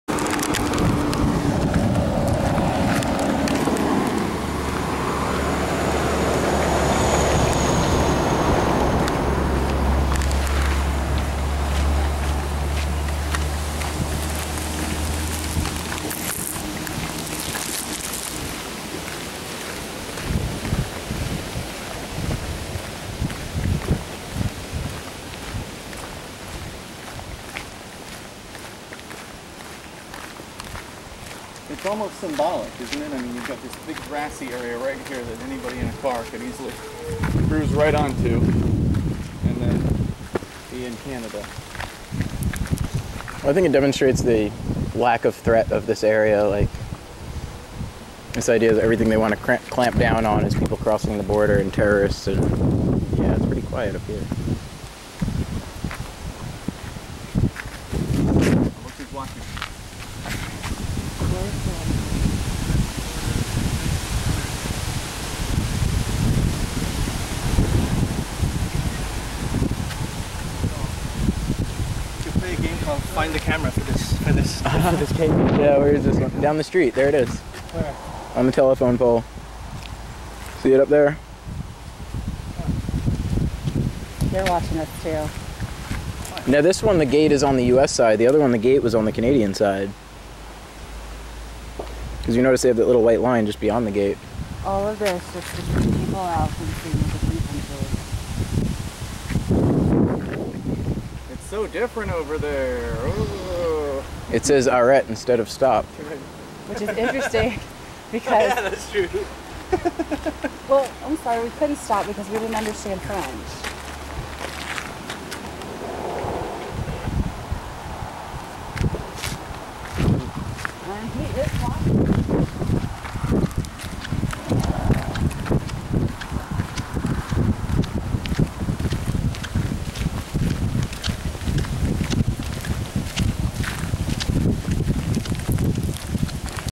2015 06 23 Road-Only Gate Obstruction, US-CA
A gate blocking off a road between Derby Line, VT and Stanstead, Quebec, featuring warnings against crossing either area. The technical line is marked on the Canada portion of the road, before the physical gate.